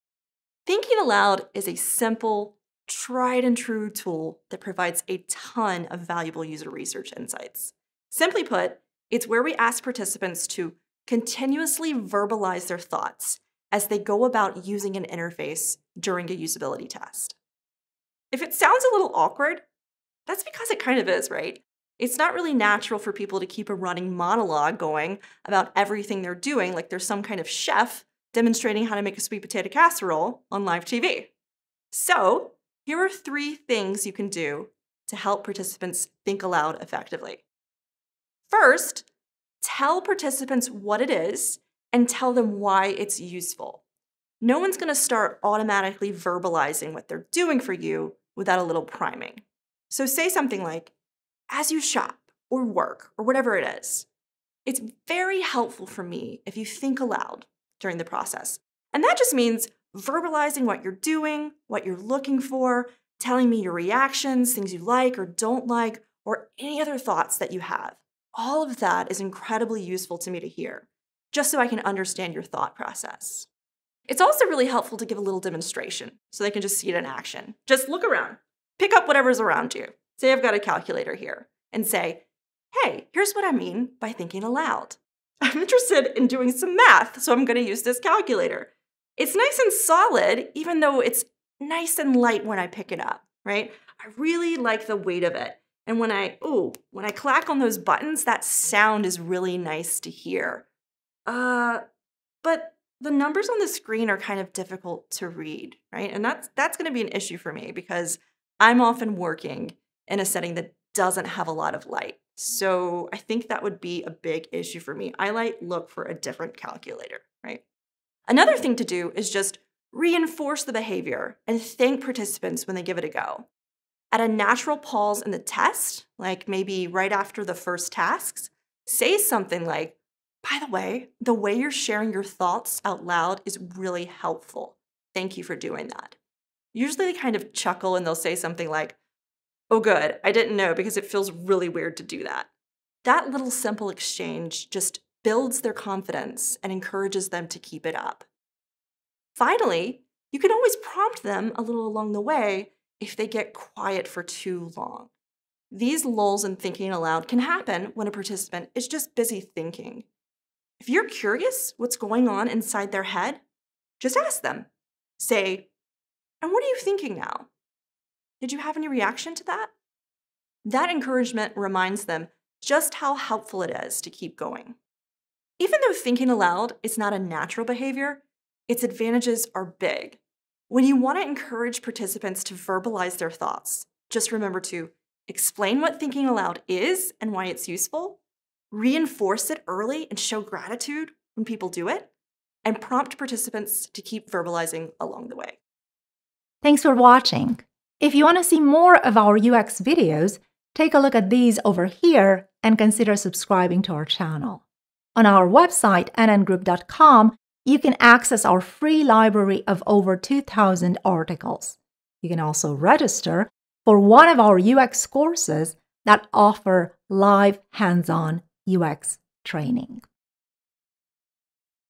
Help Users Think Aloud
Help users think aloud by explaining what it is and why it's useful, reinforcing the behavior, and prompting along the way.